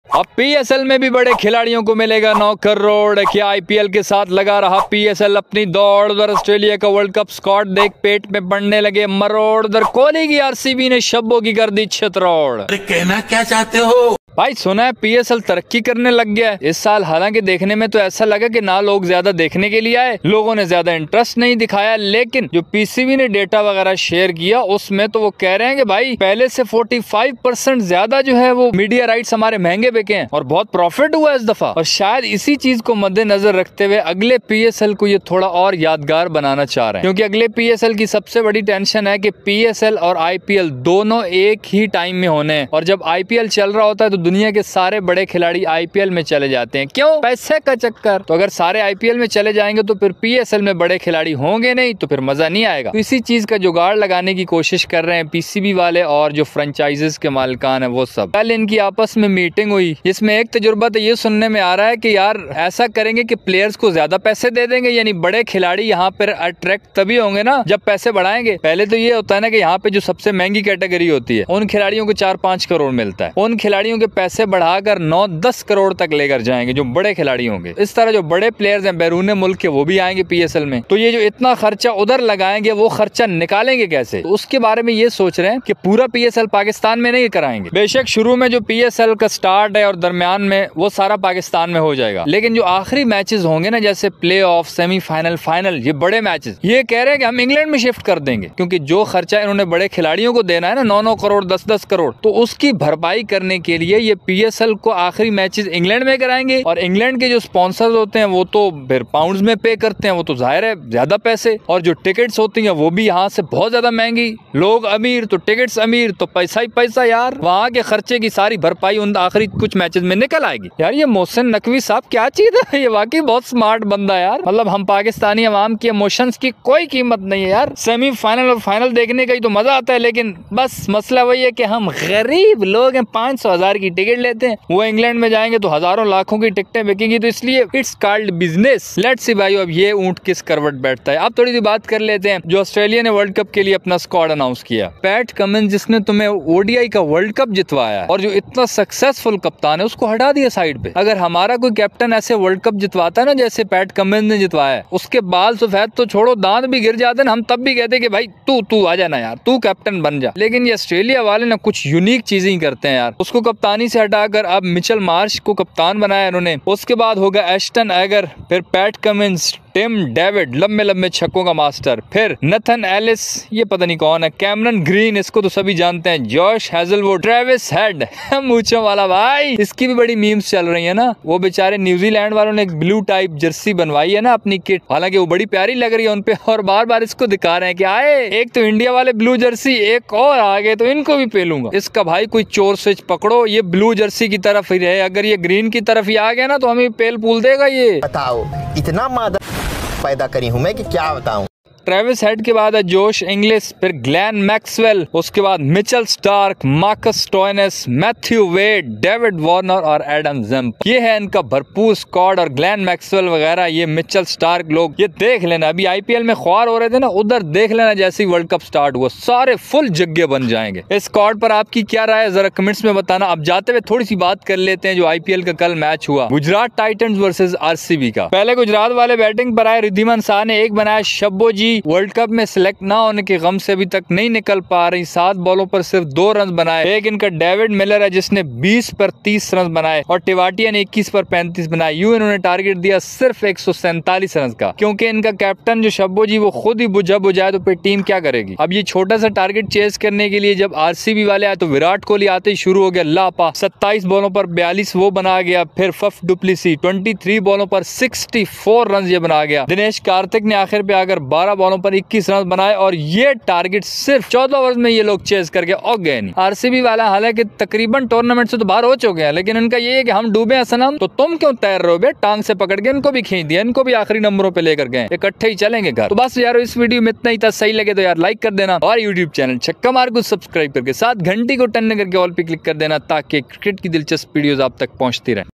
Psl players ko milega 9 crore 🤑| psl shifted England? | rcb vs Gujrat | Australia world cup squad
👉 credits